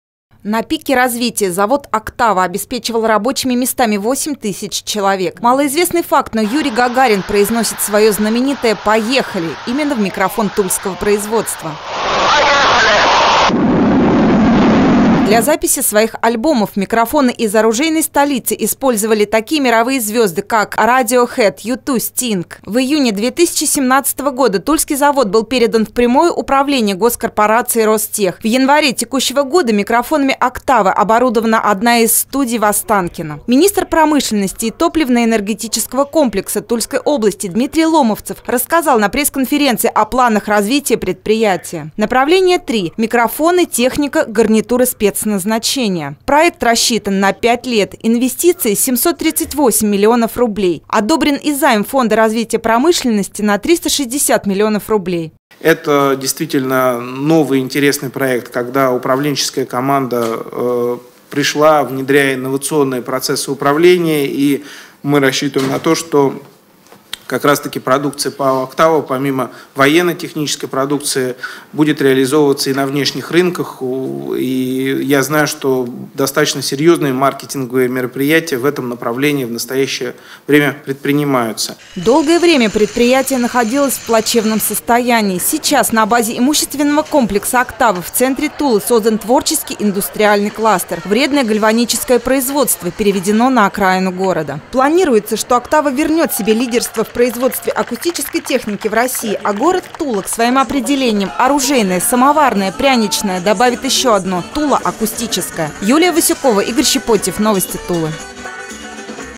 Сюжет ТСН24: Тульские микрофоны пользуются популярностью
Производство акустической аппаратуры в Туле на базе завода Октава будет расширено. О проекте  рассказал  министр промышленности и топливно-энергетического комплекса Тульской области Дмитрий Ломовцев.

Более миллиарда вложений, и 103 дополнительных рабочих места планируется создать на предприятии. А студию Останкино уже оборудуют  микрофонами тульского производства.